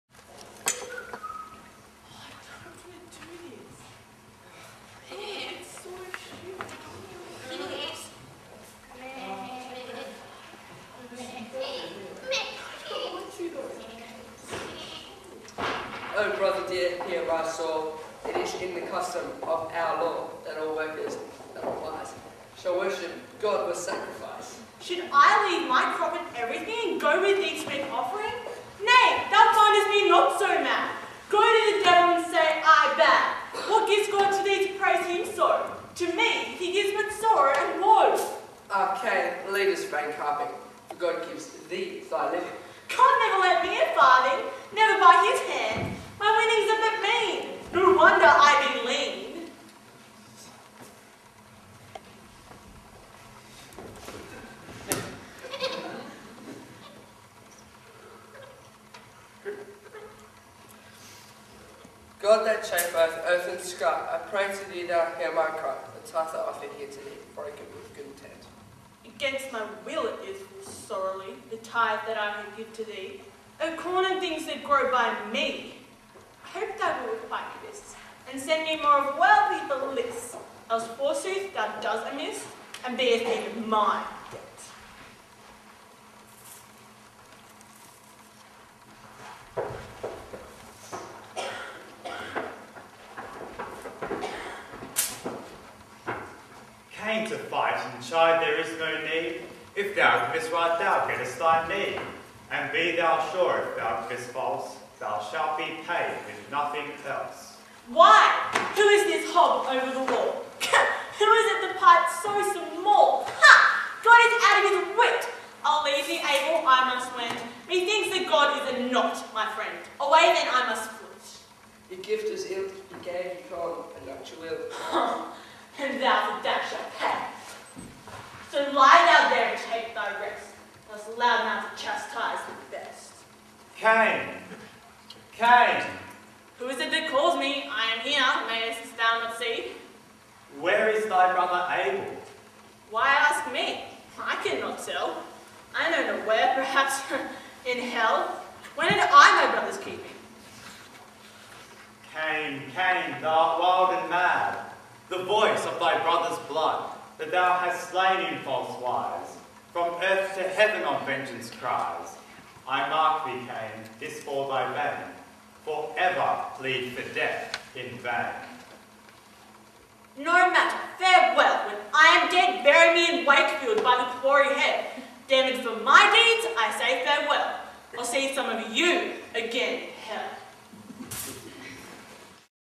Mystery Plays - Cain & Abel
Scene Three from Southern Christian College's 2011 Drama Performance - THE MYSTERY PLAYS.
Video quality not the best sorry